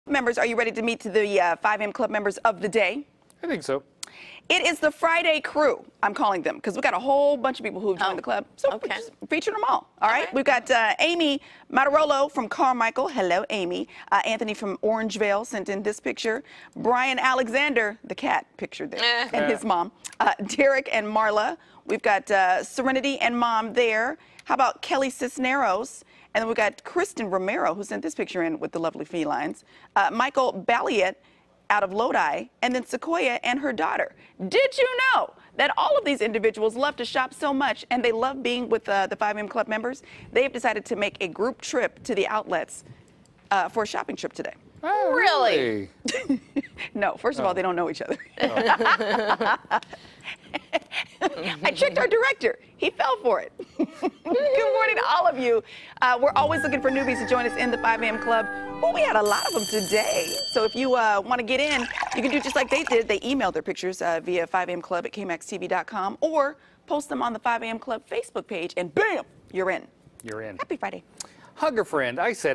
5 A.M. Club Member
Courtney announces our 5 A.M. Club Members of the day!